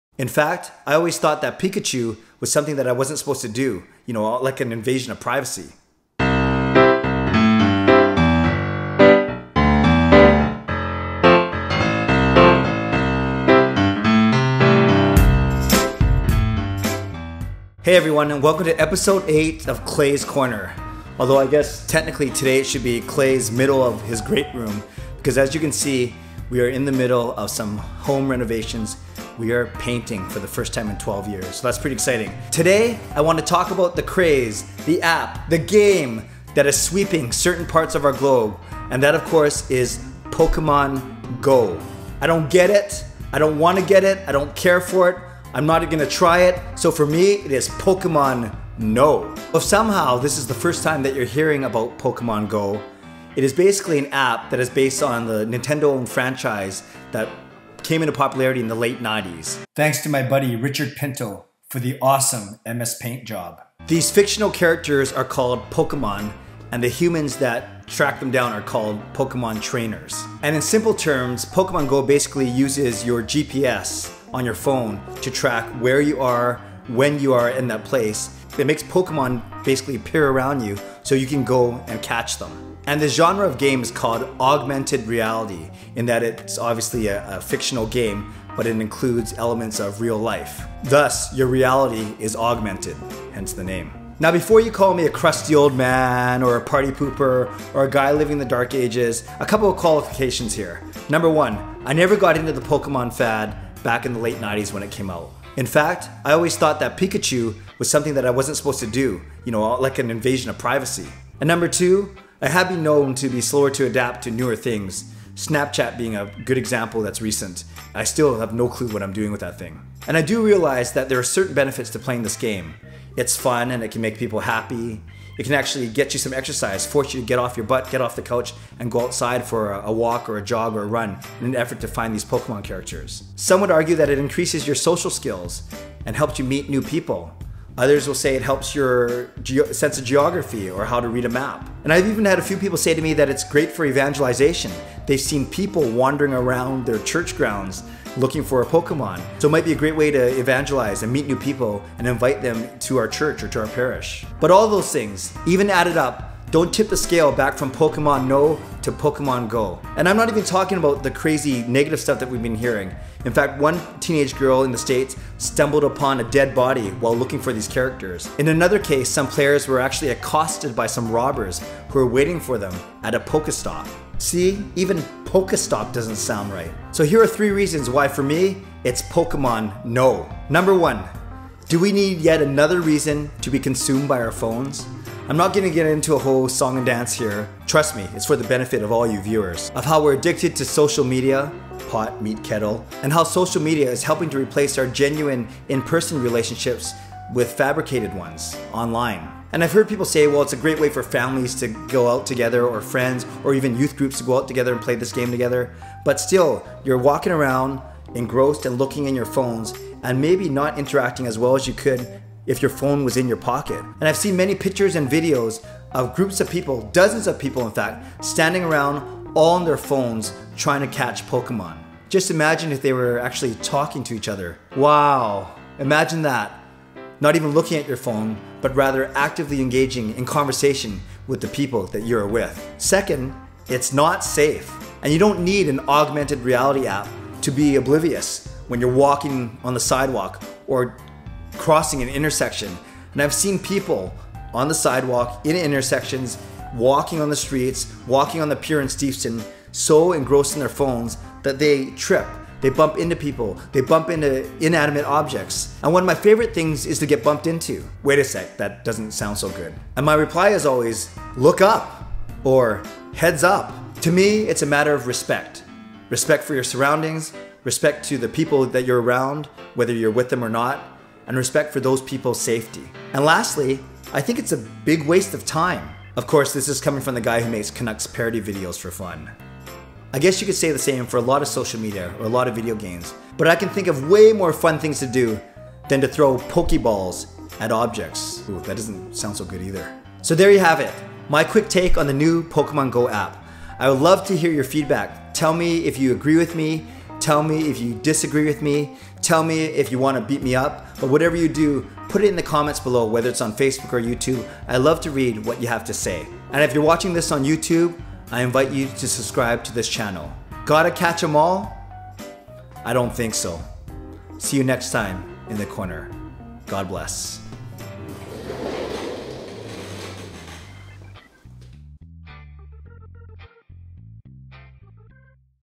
Clay's Corner Episode #8: Pokemon GO? Pokemon NO!
It's the app that's sweeping the globe: Pokemon GO.  I don't get it, I don't want to get it, and I'm not interested in trying it.

In my new Clay's Corner, I look at why for me it's Pokemon NO.